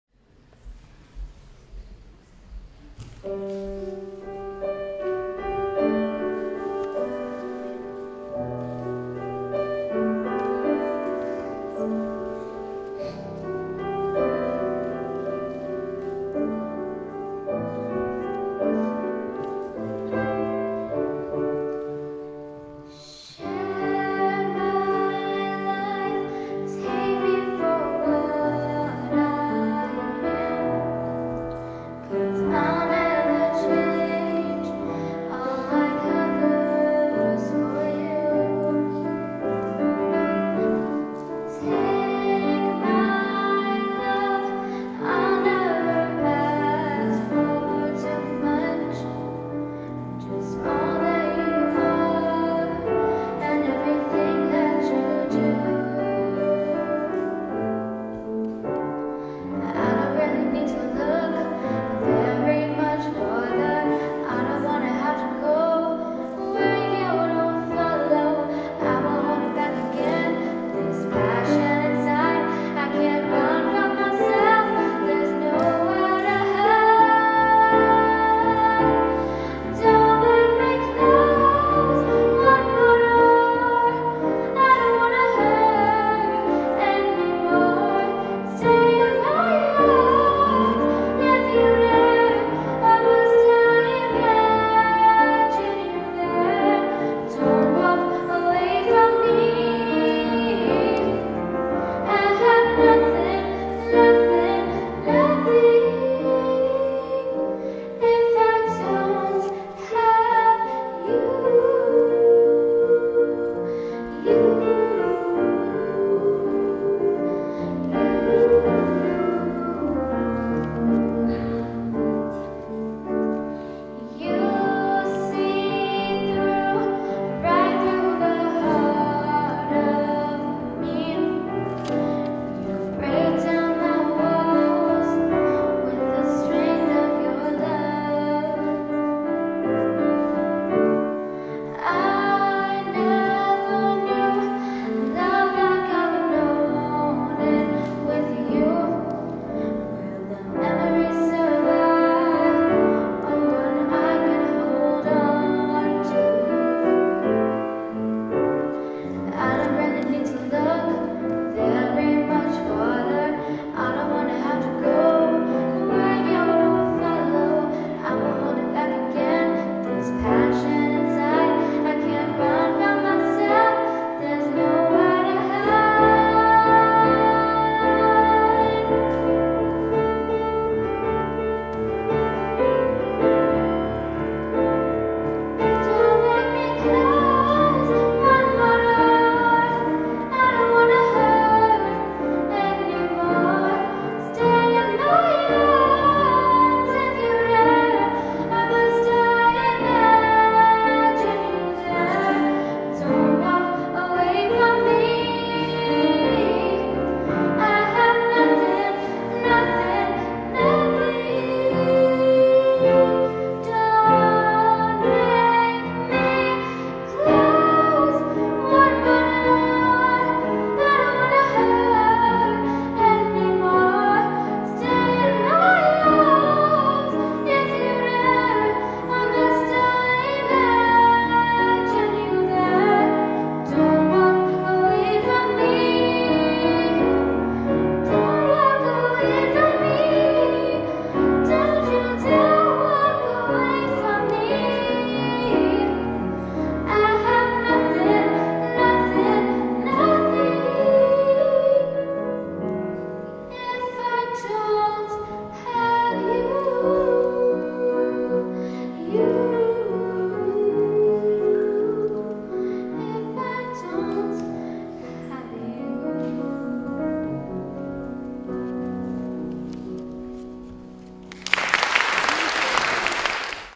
Voice Lessons: 2016 Spring Recital
Students from Veksler Academy of Music & Dance perform at Santa Clara university - December 2019 Voice Lessons in Sunnyvale, Mountain View and Milpitas provided by teachers from Veksler Academy of Music & Dance